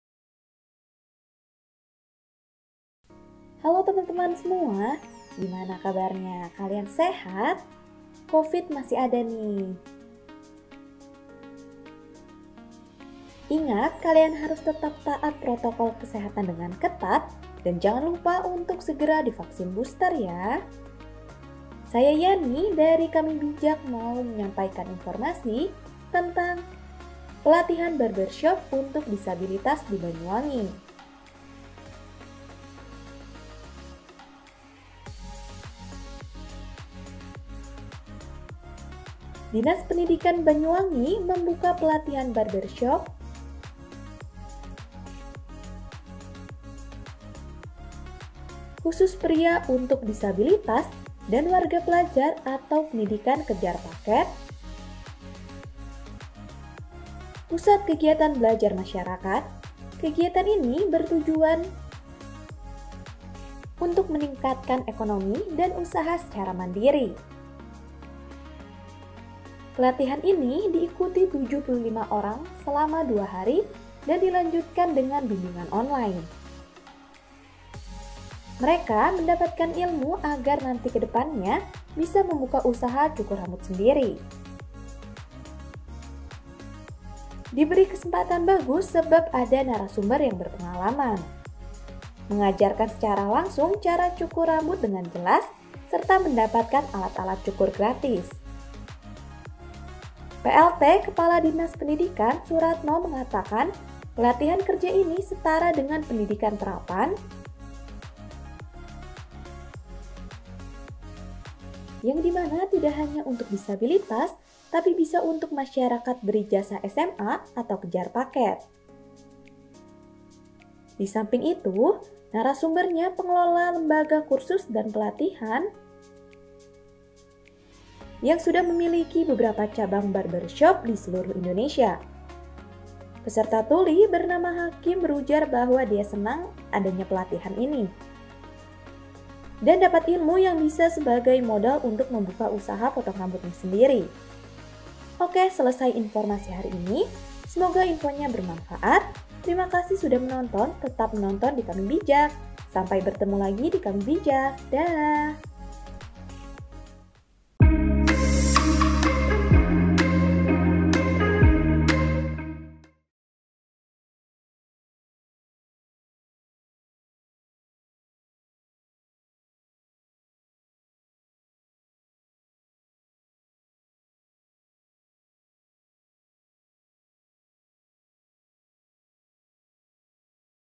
Dinas Pendidikan Banyuwangi Gelar Pelatihan Barbershop Bagi Kaum Disabilitas
Kamibijak.com, Hiburan – Agar meningkatnya kemandirian ekonomi dan mencetak wirausaha baru, terus dilakukan oleh Pemkab Banyuwangi. Saat ini, Dinas Pendidikan Banyuwangi mengadakan pelatihan keterampilan barbershop (salon untuk pria) bagi kaum disabilitas dan warga belajar (pendidikan kejar paket) Pusat Kegiatan Belajar Masyarakat (PKBBM).
Ipuk Fiestiandani, selaku Bupati Banyuwangi, mengatakan hal ini merupakan upaya untuk bisa mandiri secara ekonomi. Ternyata, tidak hanya barbershop saja, sebelumnya sudah ada banyak pelatihan yang dilakukan, salah satunya adalah menjahit.

Ia berharap kegiatan pelatihan ini bisa menjadi bekal sendiri untuk mereka membuka usaha, sekaligus untuk memulihkan perekonomian setelah adanya wabah Covid-19.

"Ke depan kami akan terus mengintensifkan pelatihan keterampilan kerja semacam ini untuk mencetak wirausahawan baru di Banyuwangi. Ini semata-mata mendukung pemulihan ekonomi warga pasca-pandemi," ujar Ipuk.

Pada kegiatan tersebut diikuti oleh 75 peserta warga belajar Pusat Kegiatan Belajar Masyarakat (PKBM), masyarakat umum, dan kaum disabilitas. Semuanya begitu antusias dalam kegiatan yang digelar pada 13-14 Juni 2022 itu.

Plt Kepala Dinas Pendidikan, Suratno mengatakan pihaknya sengaja memilih pelatihan barbershop ini karena selain mudah dilakukan juga memberikan peluang yang menjanjikan. Hal ini tentunya terlihat dari tren potong rambur di barbershop yang terus meningkat.

Selama dua hari tersebut, peserta tentunya diberikan ilmu dan lika-liku menjalankan usaha barbershop, kemudian diberi kesempatan juga untuk praktik secara langsung yang didampingi oleh narasumber berpengalaman, dan tentunya diberikan perlengkapannya. Setelah acara tersebut juga, mereka tetap diberi bimbingan secara online.

Hakim, peserta Tuli mengaku senang dengan acaranya pelatihan ini. Karena ilmunya bisa menjadi modal bagi dirinya untuk membuka usaha potong rambut sendiri.
"Saya senang ada pelatihan barbershop. Ilmunya bisa jadi modal kami memulai usaha cukur rambut," ujar Hakim dengan menggunakan bahasa isyarat. (MG/Alissa)